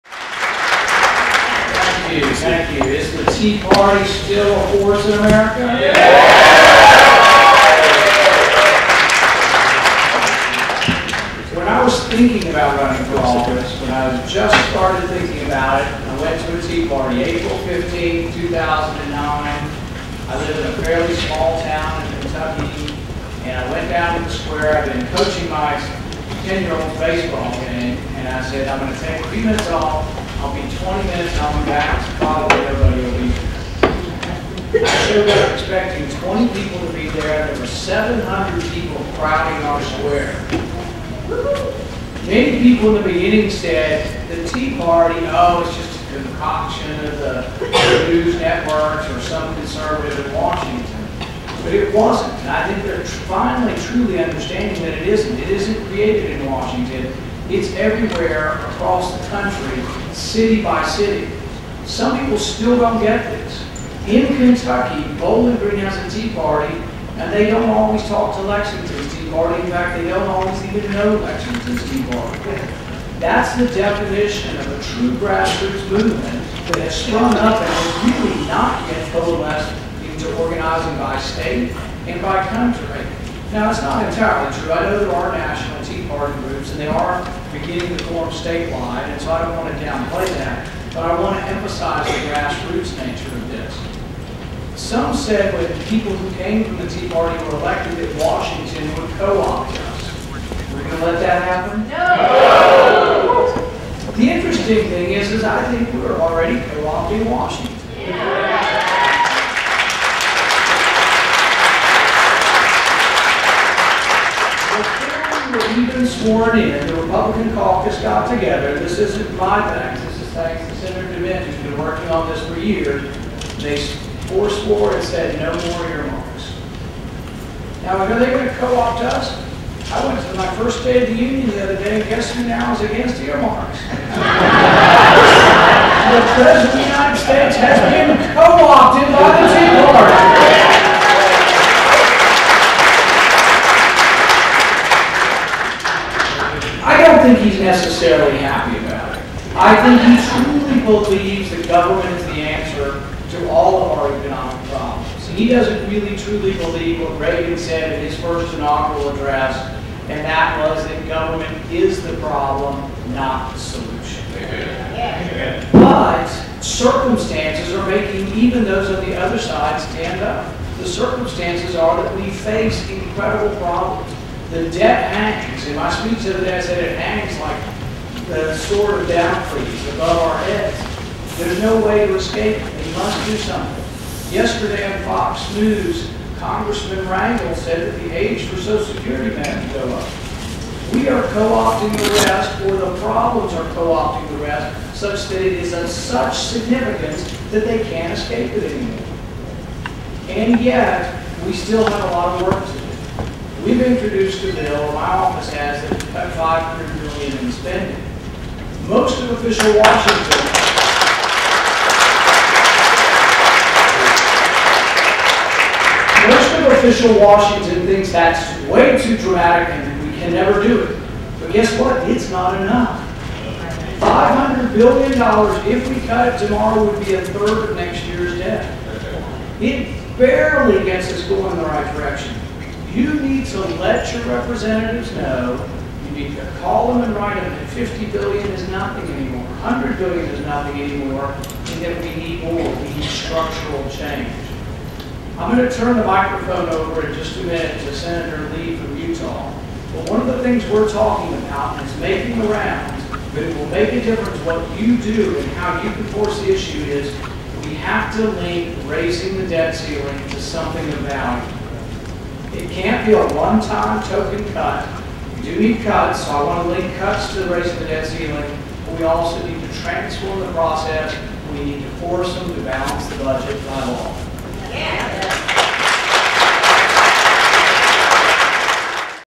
Rand Paul: U.S. Senate Tea Party Caucus 01/27/11
WASHINGTON [Hart Senate Office Building] 01/27/11 - Sen. Rand Paul (KY) addresses the crowd at the first ever U.S. Senate Tea Party Caucus meeting.  Topics of interest included the grassroots nature of the Tea Party movement, the changing political dynamic in Washington D.C., and the challenges facing the Tea Party on the road ahead.